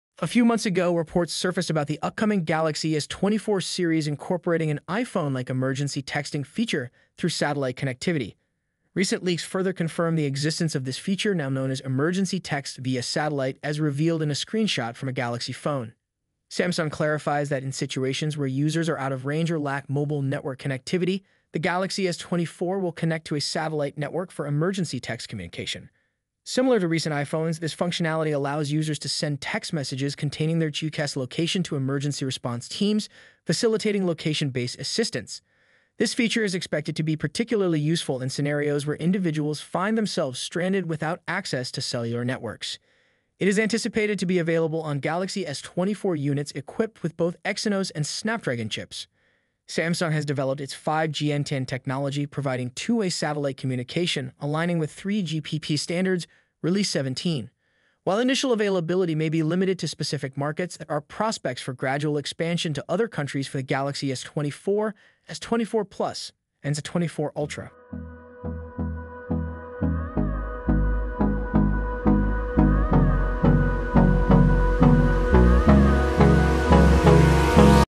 Samsung Galaxy S24 Series - WOW! Satellite Emergency Texting & 5GN10 Tech REVEALED!
Some months back, there were rumors about the Galaxy S24 series having a feature similar to the iPhone's emergency texting via satellite. Screenshots from a Galaxy phone showed that the Galaxy S24 could connect to a satellite network for emergency text communication.

This feature allows patients to communicate with emergency response teams, enabling location-based assistance. The Galaxy S24 models with both Exynos and Snapdragon chips are equipped with Samsung's 5GN 10 technology, enabling two-way communication in line with the 3GPP standards released in 17.